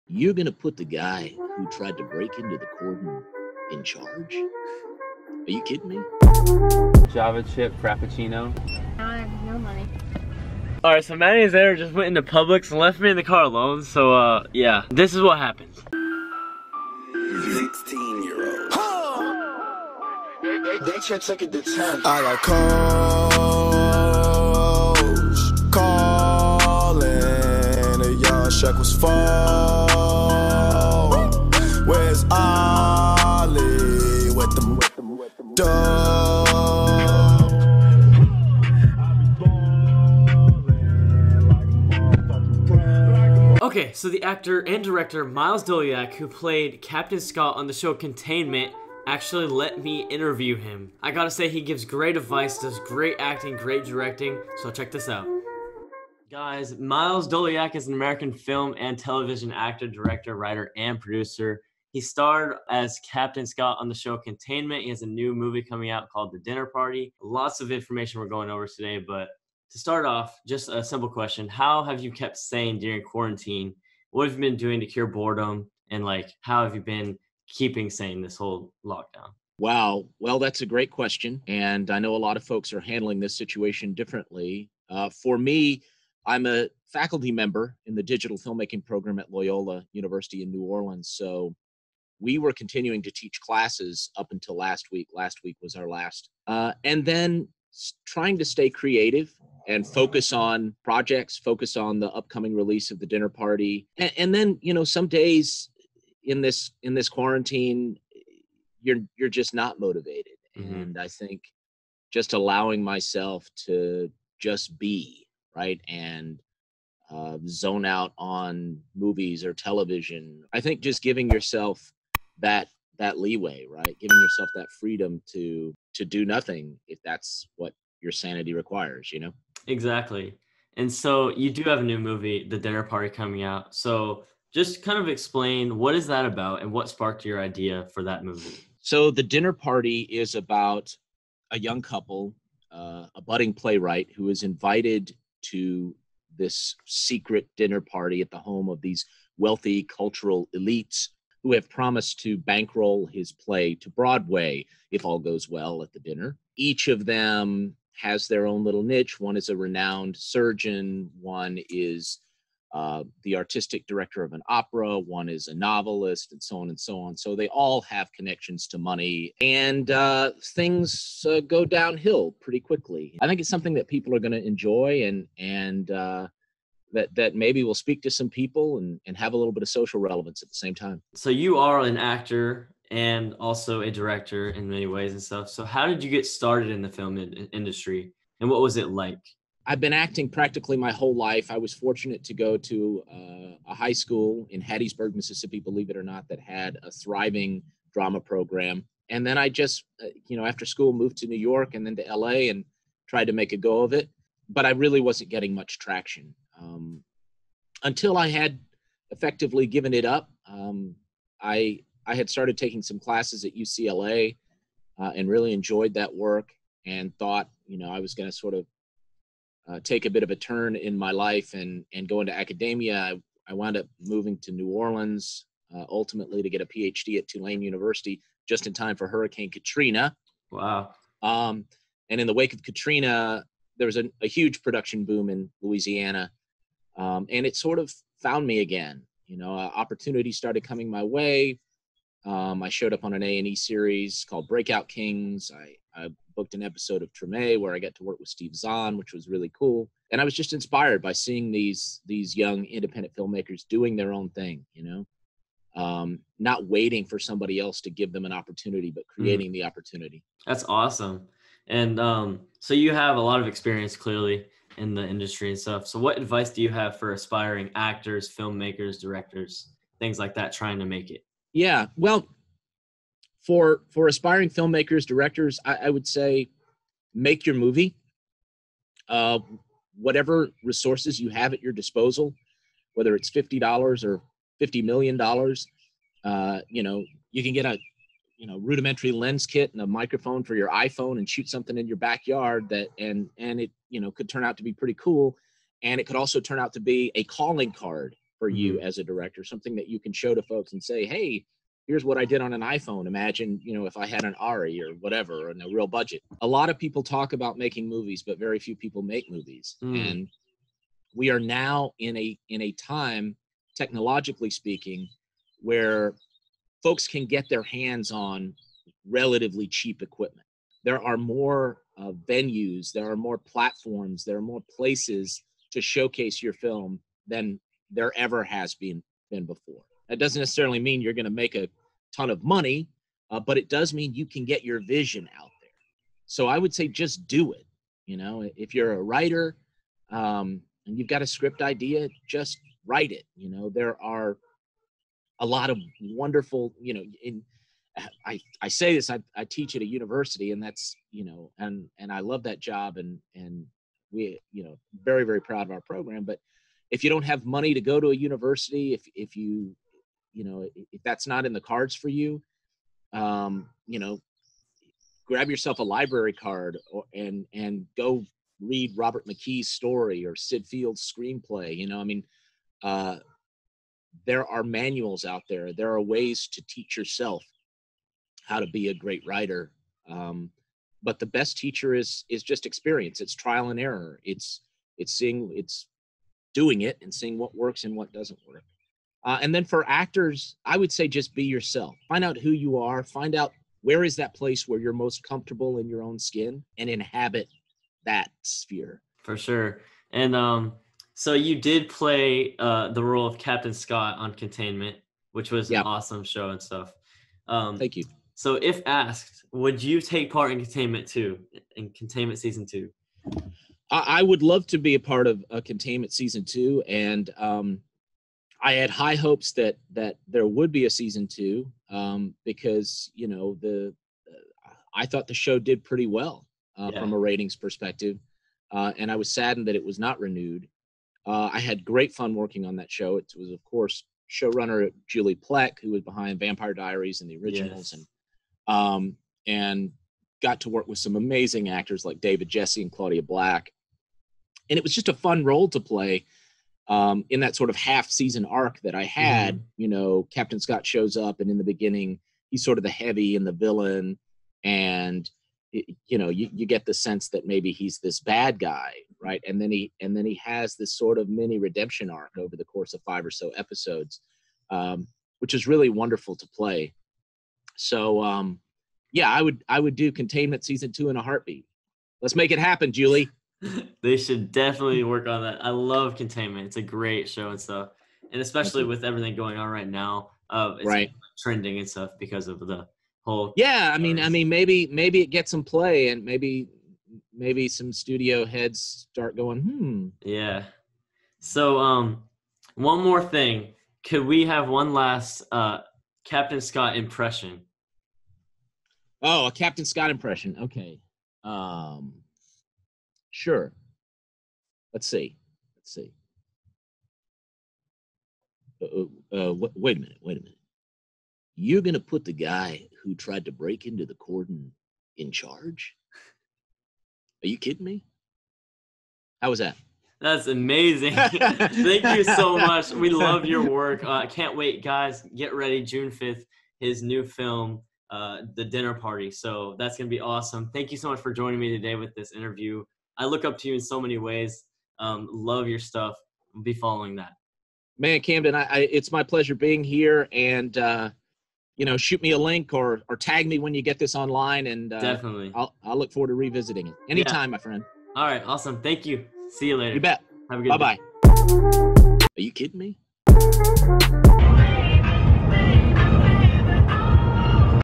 Miles Doleac's Advice for Actors and Filmmakers (INTERVIEW)
Director Miles Doleac shares his best advice to becoming a successful filmmaker, actor, and director in a noteworthy interview.

17 year old kid from Florida born to make people laugh. This is my life.